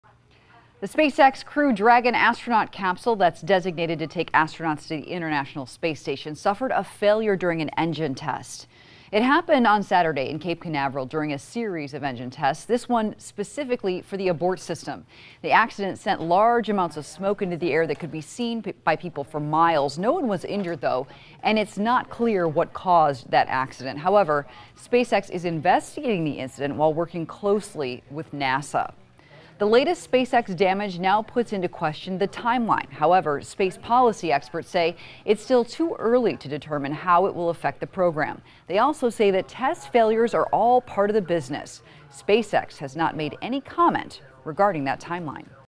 SpaceX capsule has failure during test
There's no word on how this will affect the timeline for using the capsule to launch humans into space.